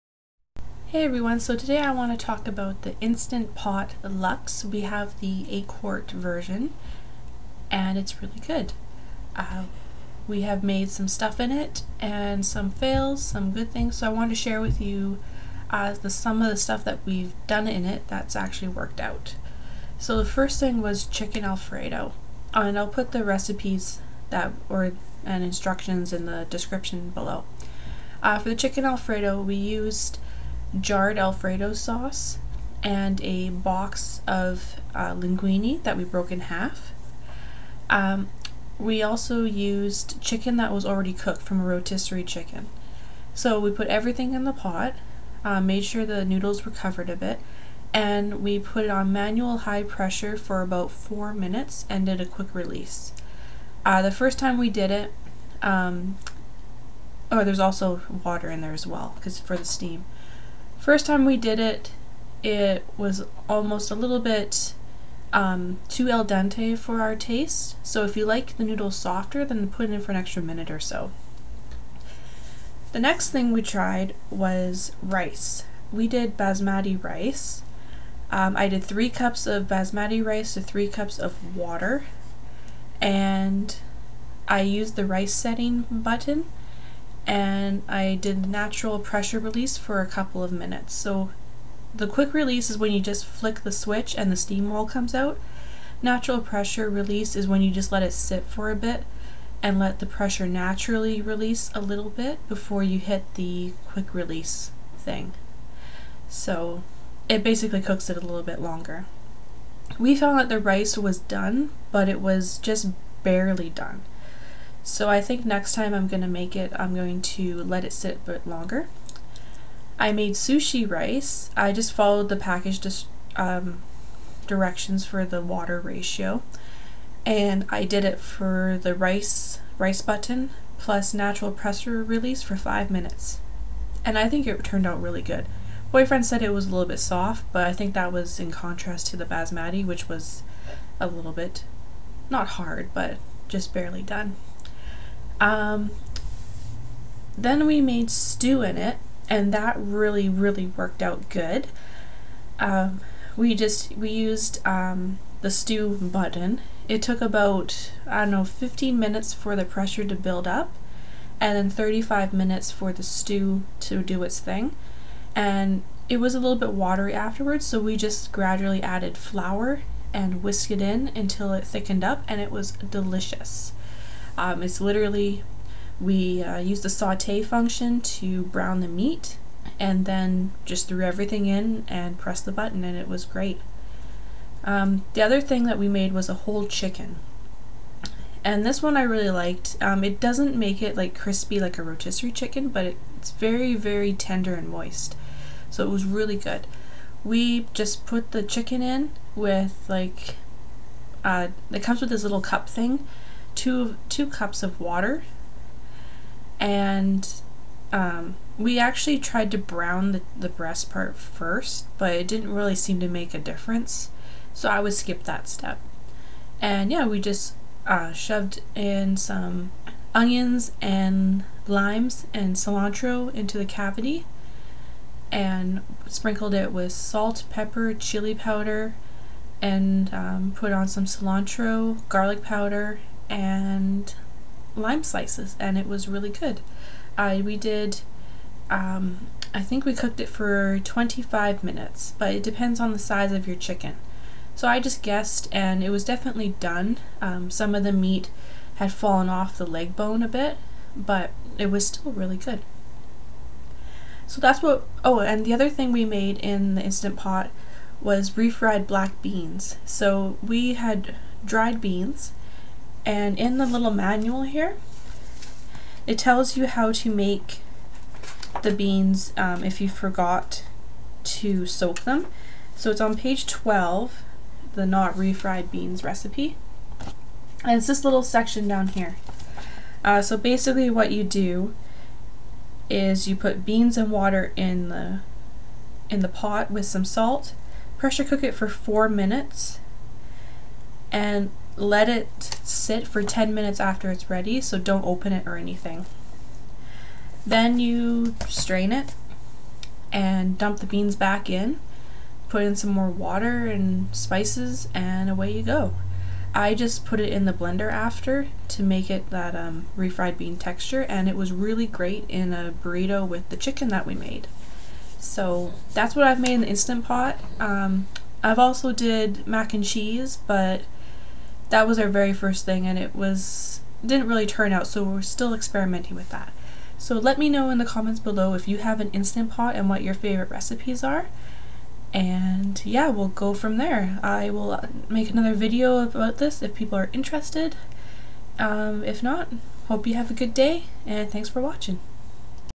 Instant Pot Lux Review/Recipes
Recipes
Whole Chicken
- whole frying chicken
- slices of lime
- 1 onion chopped into big pieces
- handful of cilantro
- spices to taste: chili powder, garlic powder, salt and pepper
manual high pressure 25 minutes, quick release
Chicken Alfredo
- 2 cups water
- 2 jars alfredo sauce
- 1 box linguine 454g
- chicken cooked already (2 cups or so)
- manual high pressure 4 minutes, quick release
Beef and Broccoli
- pack of beef stirfry (frozen)
- ½ cup soy sauce
- 2 tbsp brown sugar
- 1 cup water
- ginger
- garlic
- sliced onion
manual high 15 minutes, quick release, then add broccoli then another
5 minutes, quick release
Basmati Rice
- 3 cups basmati rice
- 3 cups water
rice setting, natural pressure release few extra mins
Sticky Rice
- package directions
- rice button, natural pressure release 5 minutes
Sean's Stew
- package of stew meat
- 3 potatoes
- 3 carrots
- 3 onions
- red wine (half a cup)
- 1 cup mushrooms sliced
- italian spices (just a little)
-pepper and salt
- water (3-4 cups depending on amount of veggies)
- paprika (just a little)
-garlic (lots)
-worchestershire sauce (lots)
Saute function, brown the meat then add in everything else.
Stew function, quick pressure release
Forgot to Soak Beans
- 2 cups black beans, dry
- 8 cups of water
- 1 tsp salt
manual high 4 minutes, natural pressure release for 10 minutes

Photography & Graphic Editing: Myself (Crystal)
Thumbnails may contain images that appear to be book covers. These are photographs taken by myself of books that I own.